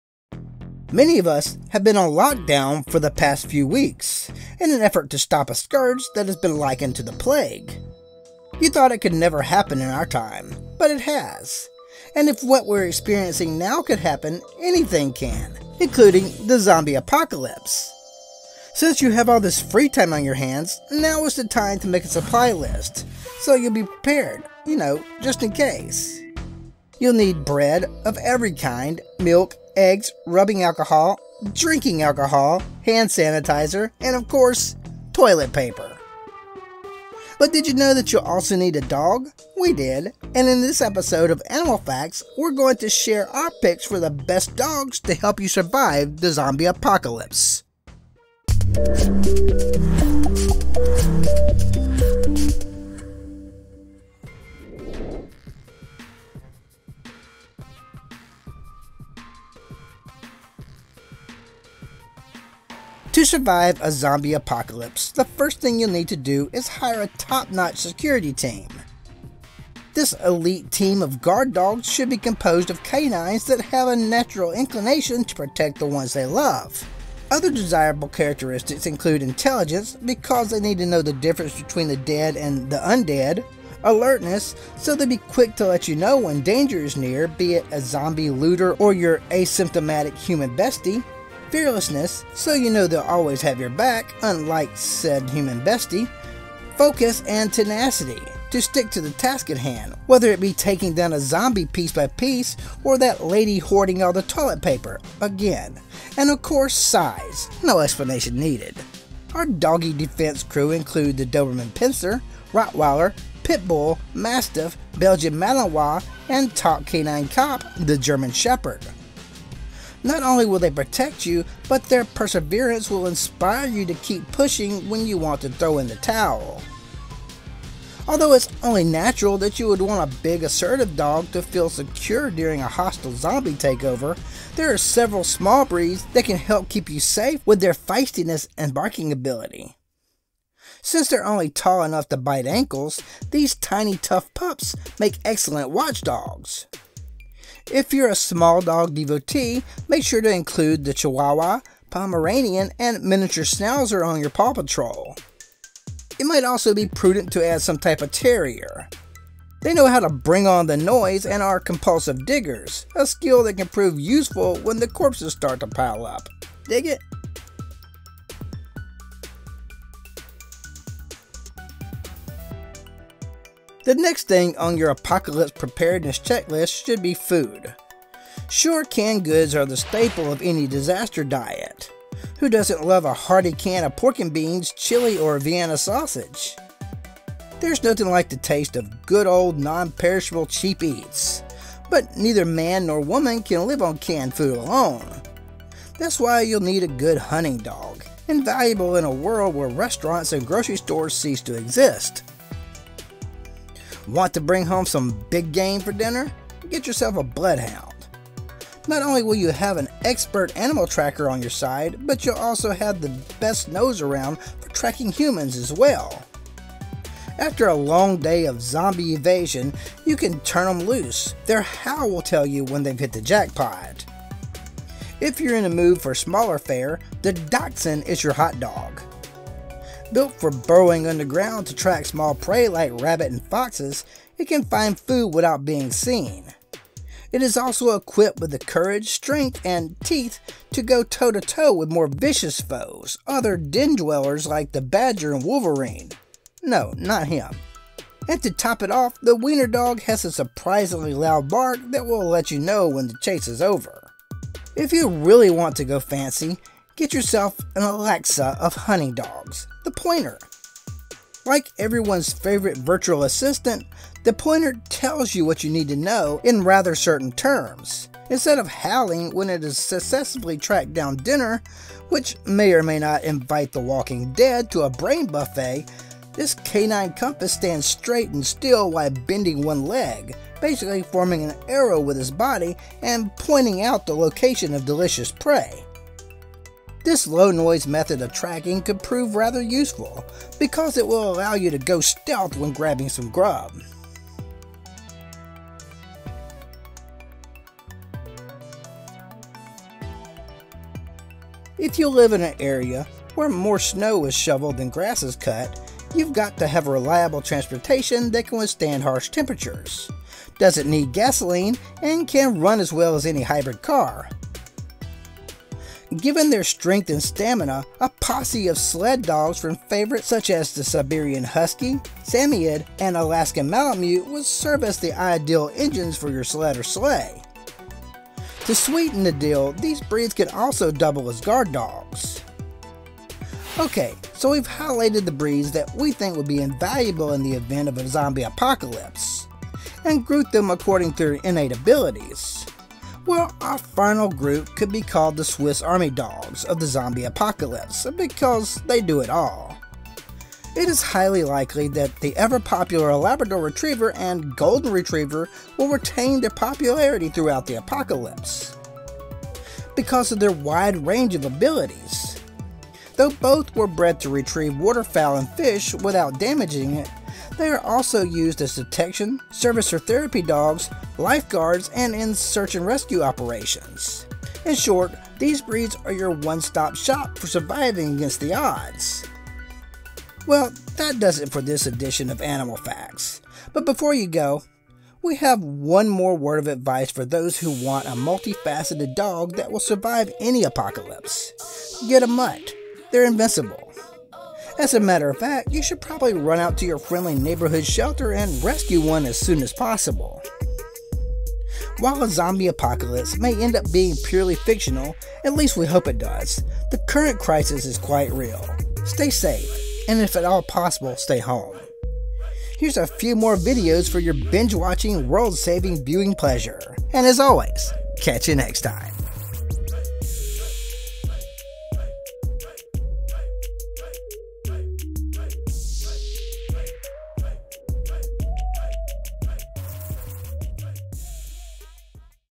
Best Dogs for Zombie Apocalypse - Dogs 101
Best Dogs for Zombie Apocalypse - Dogs 101 Are you prepared for the zombie apocalypse? It's always better to be safe than sorry, so start making your supply list now! In this episode of Animal Facts, we'll tell you which dogs you need on your team to survive the end of the world. From guard dogs to hunting dogs, we've got you covered. Plus, we'll reveal the ultimate Swiss Army Dogs that can do it all. And don't forget, mutts are always a great option too! Stay safe and informed with Animal Facts.

0:00 - Introduction and Can Dogs Survive a Zombie Apocalypse?
1:03 -Best Guard Dogs for Zombie Apocalypse
2:07 - Small Breeds
3:02 - Hunting Dogs
5:01 - Sled Dogs
5:48 - Swiss Army Dogs
6:33 - The Power of Mutts
6:49 - Conclusion